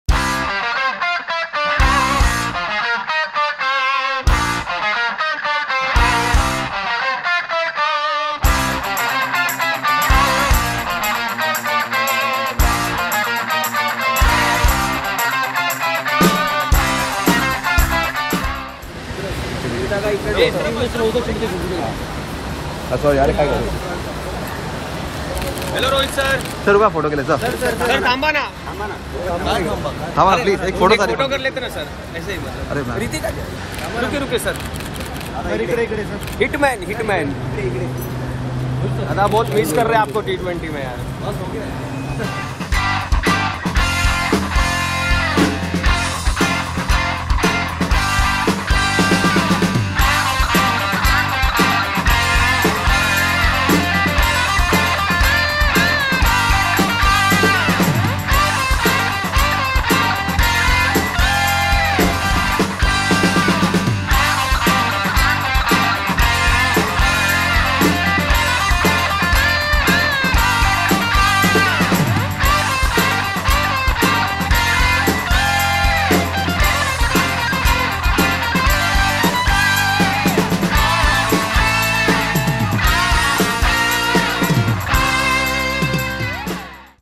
छोटे ब्रेक पर निकले कप्तान Rohit Sharma, एयरपोर्ट पर फैंस ने घेरा, देखिए मजेदार वीडियो | Team India
Captain Rohit Sharma is currently on a break from cricket. Now the audience will see him playing directly in the Test series against New Zealand. When fans saw him at the airport with his wife Ritika, there was a competition for photos.

कप्तान रोहित शर्मा ने फिलहाल क्रिकेट से ब्रेक पर हैं. अब उन्हें सीधे न्यूजीलैंड के खिलाफ टेस्ट सीरीज में दर्शक खेलते हुए देखेंगे. उन्हें पत्नी रितिका के साथ एयरपोर्ट पर फैंस ने देखा तो फोटो के लिए होड़ लग गई.